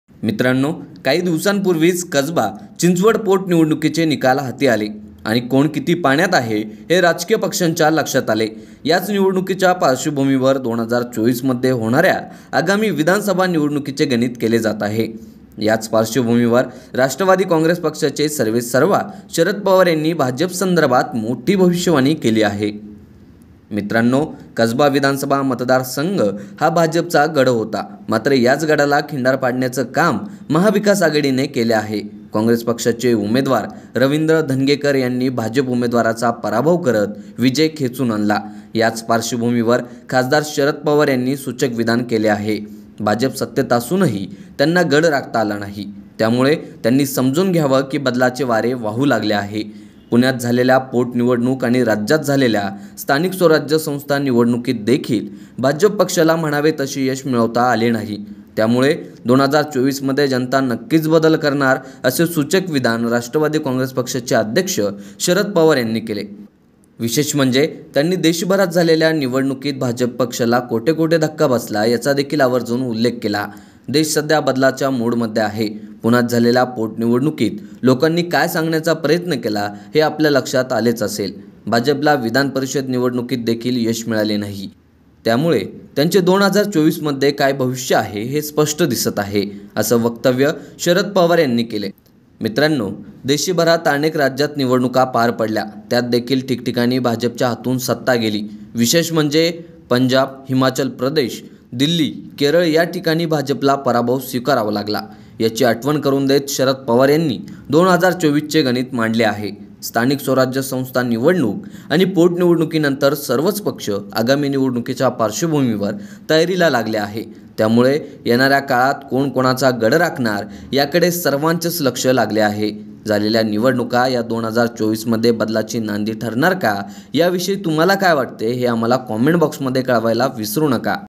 शरद पवारांचे सूचक विधान, 2024 ठरणार बदलाची नांदी | Sharad Pawar | BJP | Maybhumi Political
मित्रांनो, काही दिवसांपूर्वीच कसबा चिंचवड पोट निवडणुकीचे निकाल हाती आले आणि कोण किती पाण्यात आहे. हे राजकीय पक्षांच्या लक्षात आले. याच निवडणुकीच्या पार्श्वभूमीवर २०२४ मध्ये होणाऱ्या आगामी विधानसभा निवडणुकीचे गणित केले जात आहे. याच पाश्र्वभूमीवर राष्ट्रवादी काँग्रेस पक्षाचे सर्वेसर्वा शरद पवार यांनी भाजप मोठी भविष्यवाणी केली आहे.